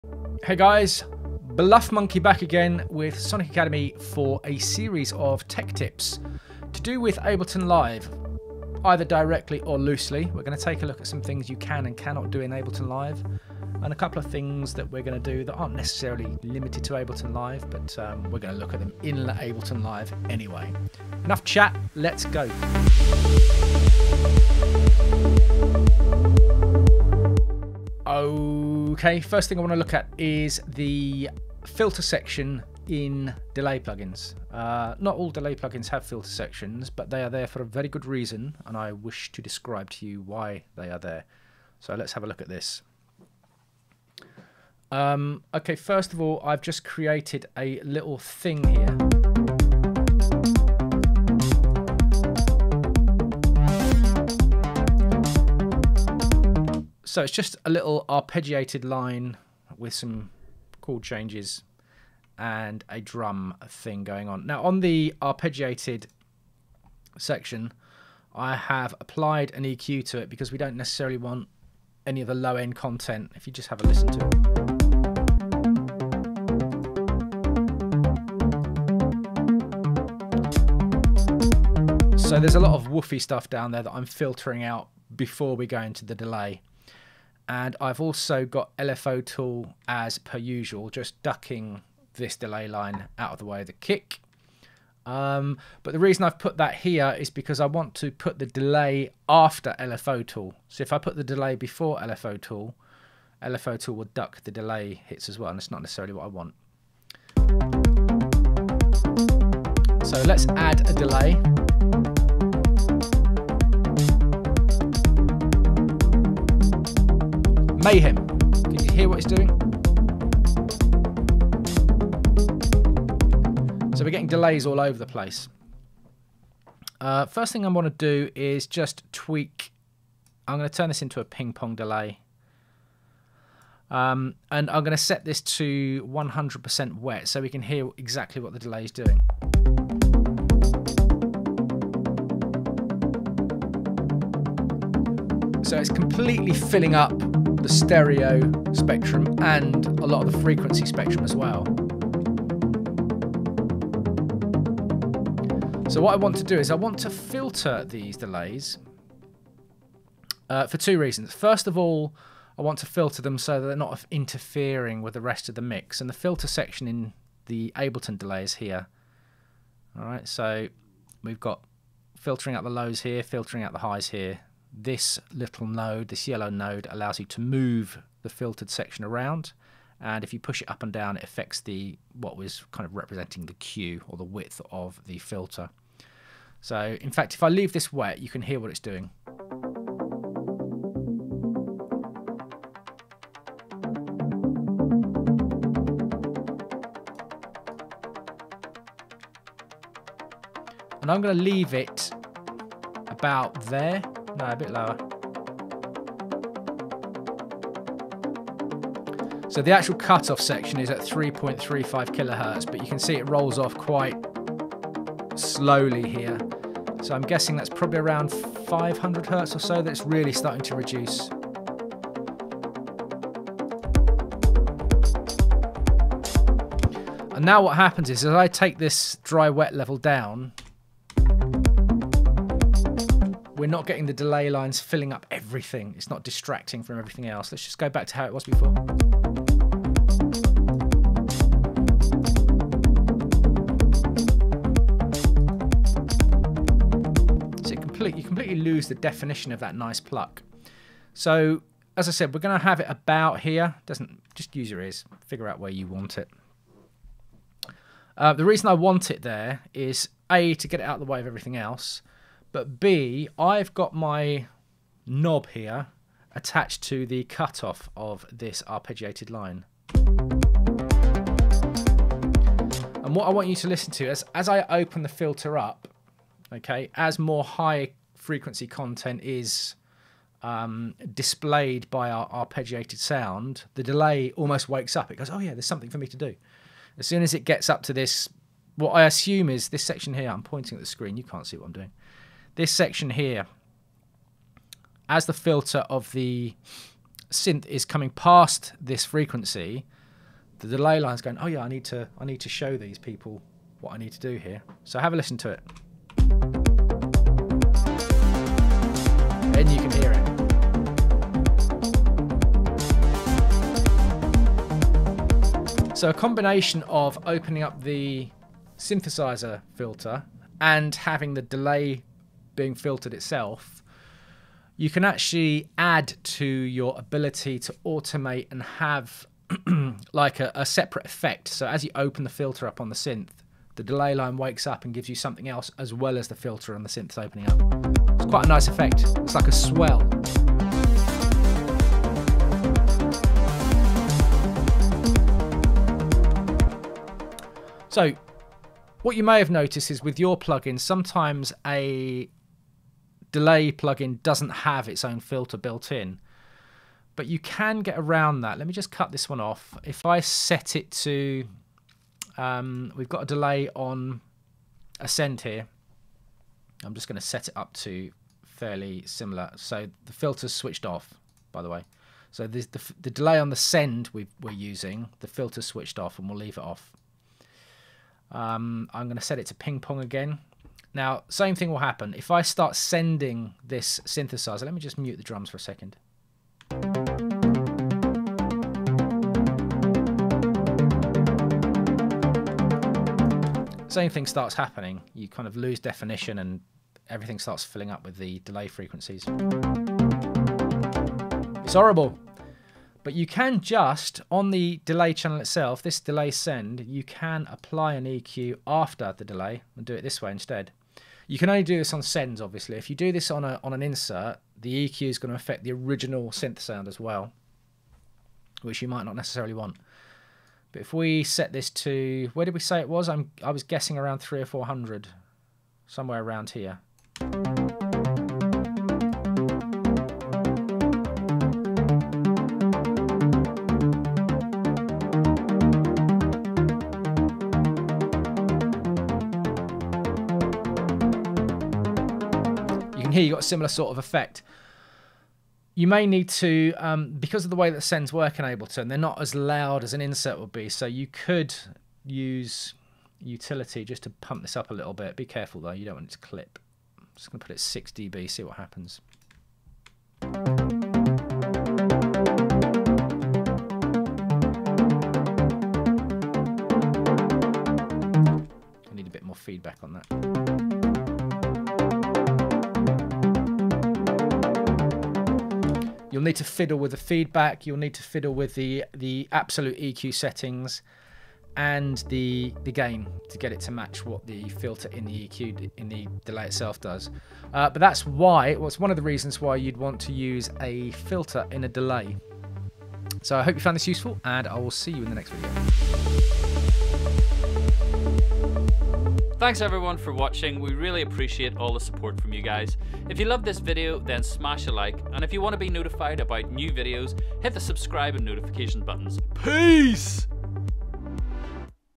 Tech Tips Vol.67 - Ableton Secrets
It's time for another set of Tech Tips as we welcome back Bluffmunkey for Volume 67.

Over these ten videos, he's checking out some tips and tricks in Ableton that'll help you speed up workflow so you can get on with the fun stuff!

We look at key mapping shortcuts, organisation tips and setting up your track's default along with making delays sound better, unusual wavetables, levelling up on your saturator use and more - there's something for all Ableton users here, whatever your ability.

[ Video Chapters ]
00:00 Introduction
00:30 Delay Filter